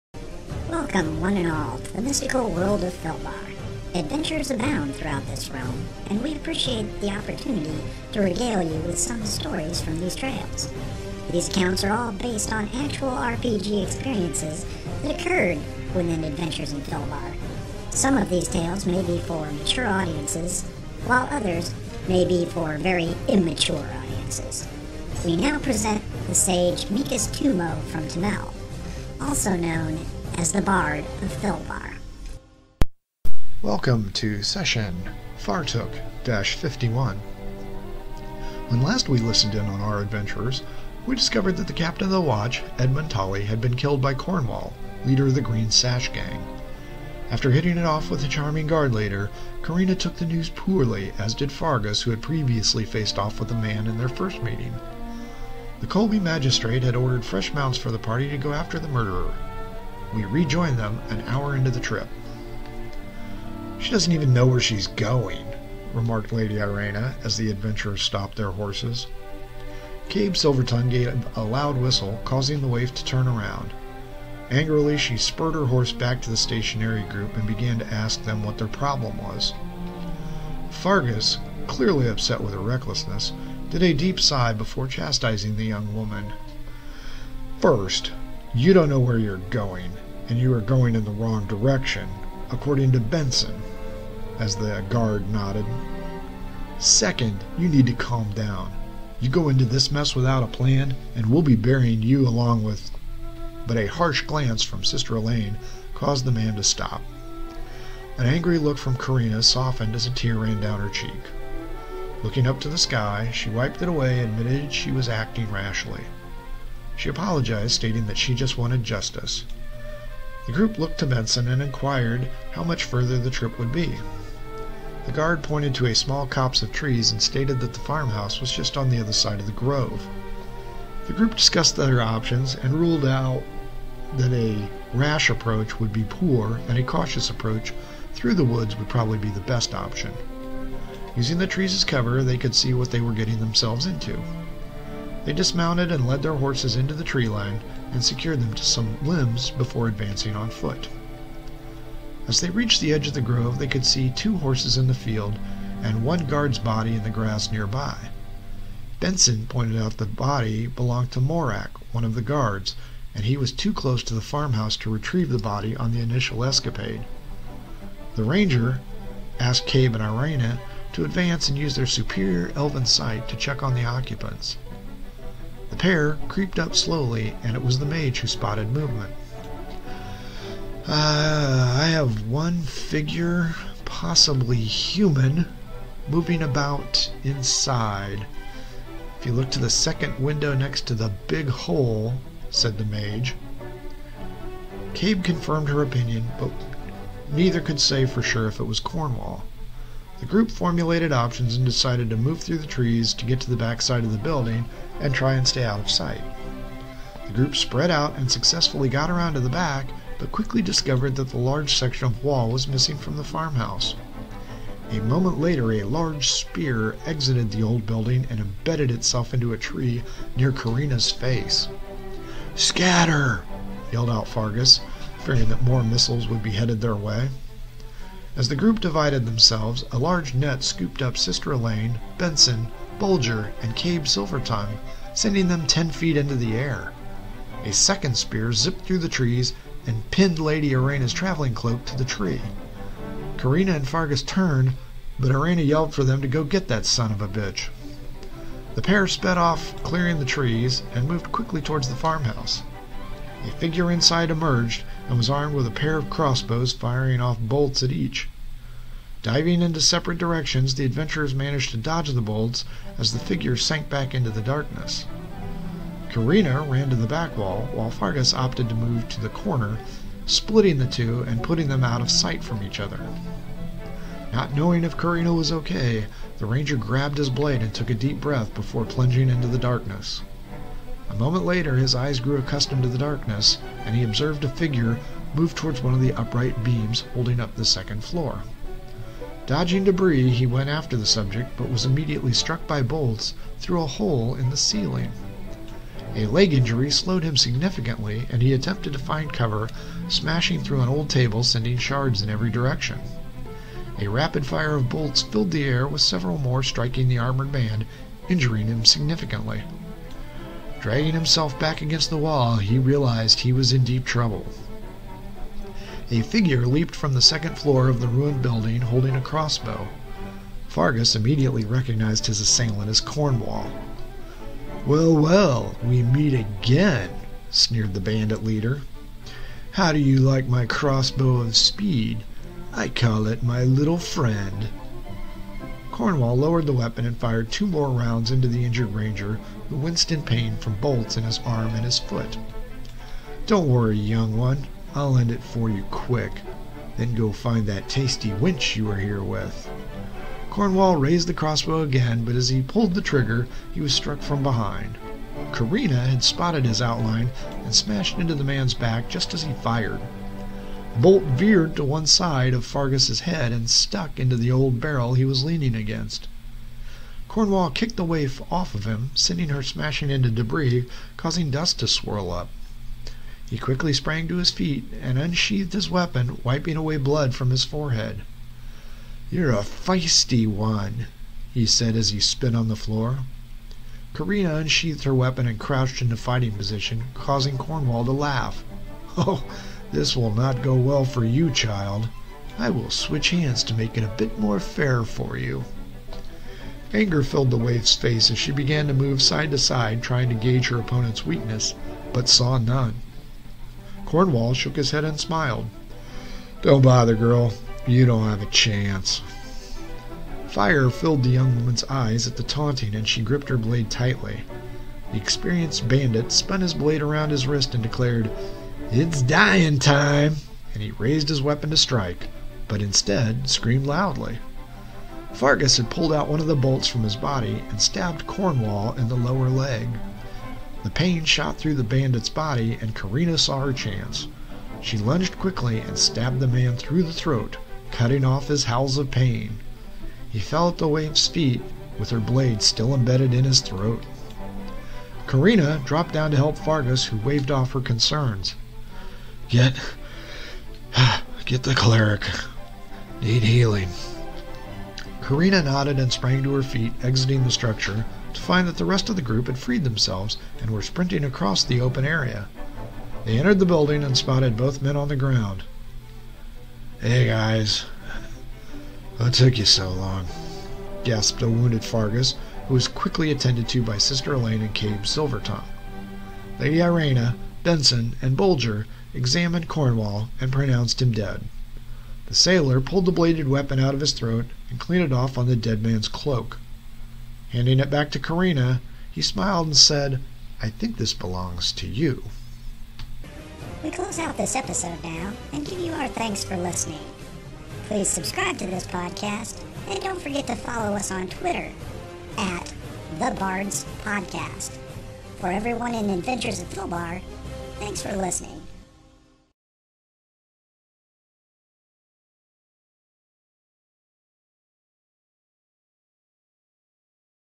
Session Fartook - 51 Cornwall
The party seeks revenge at the abandoned farmhouse. Finding Cornwall they get more than they bargained for...